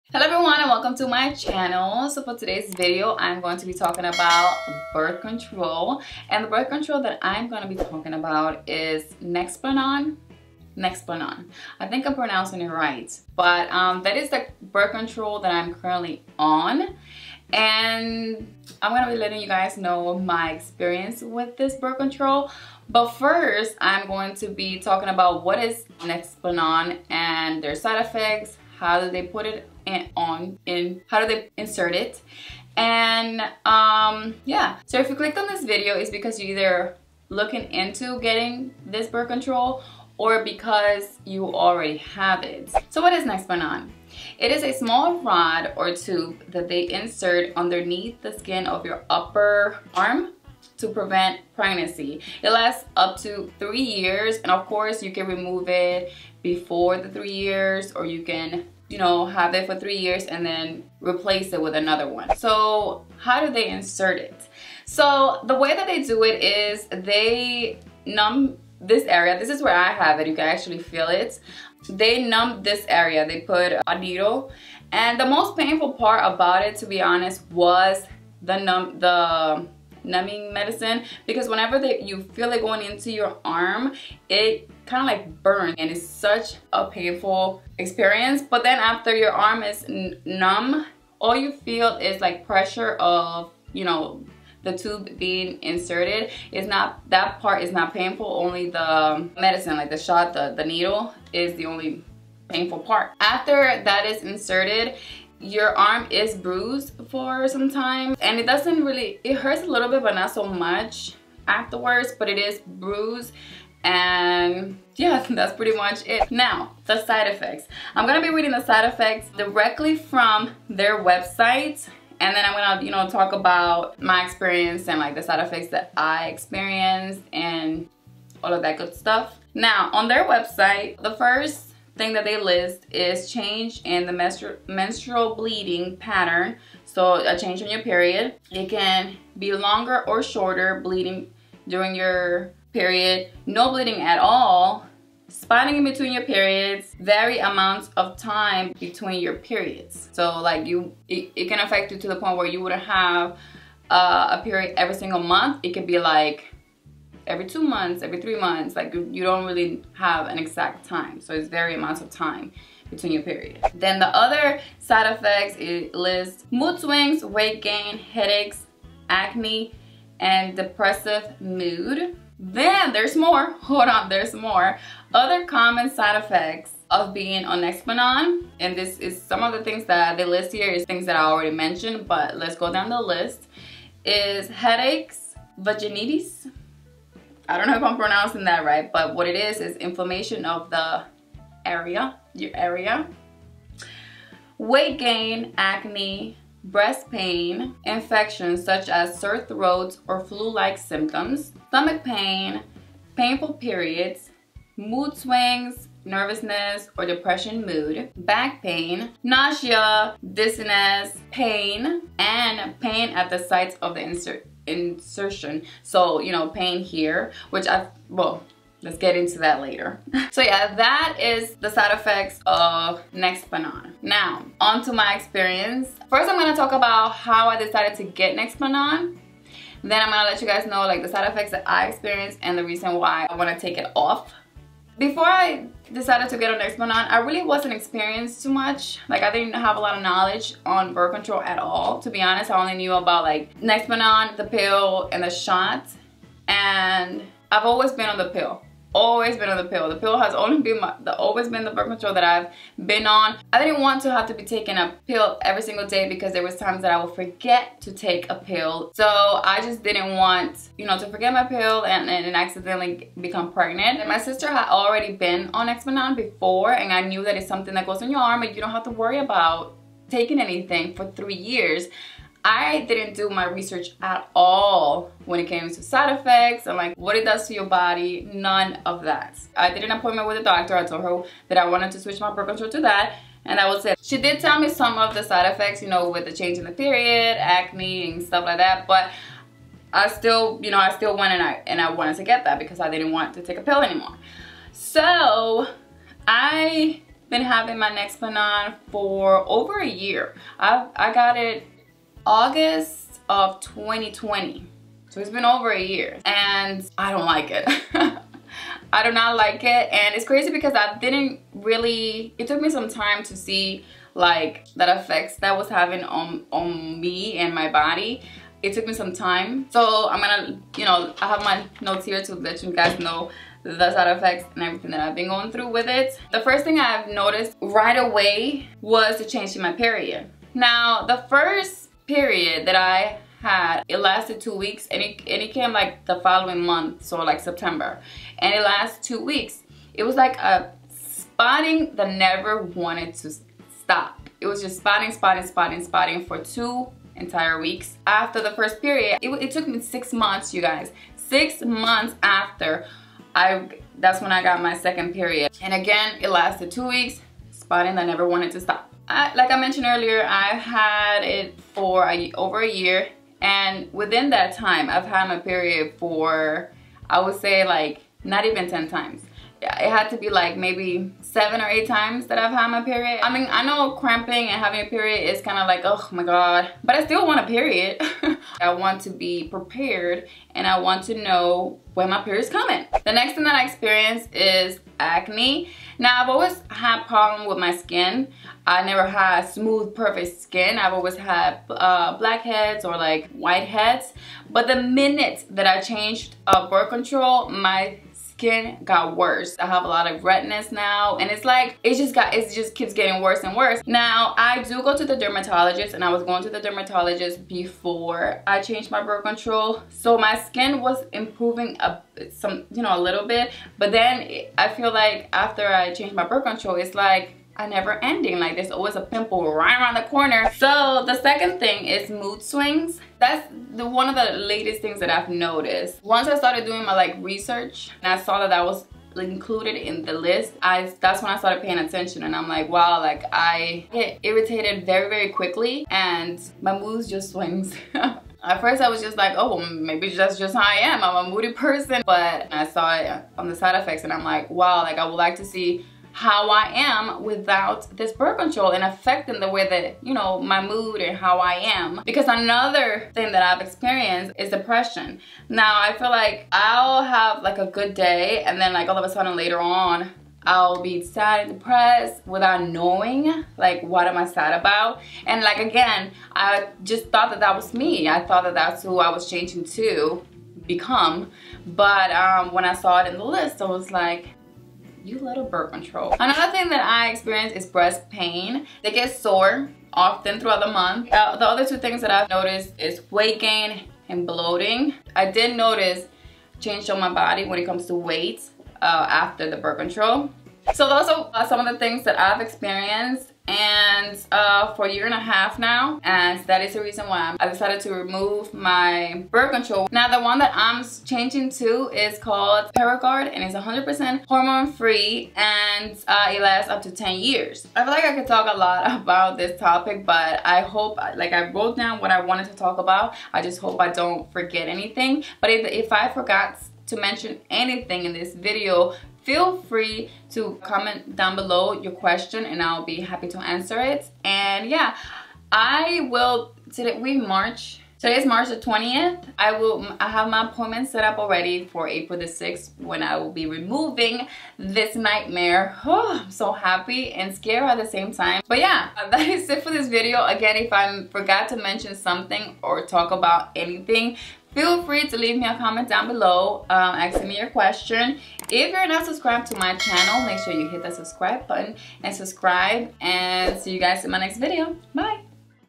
Birth Control Implant Experience | Nexplanon Implant Side Effects
WATCH THIS before getting Nexplanon Birth Control. In this video I share my experience with Nexplanon birth control and what birth control did to my body and mental health.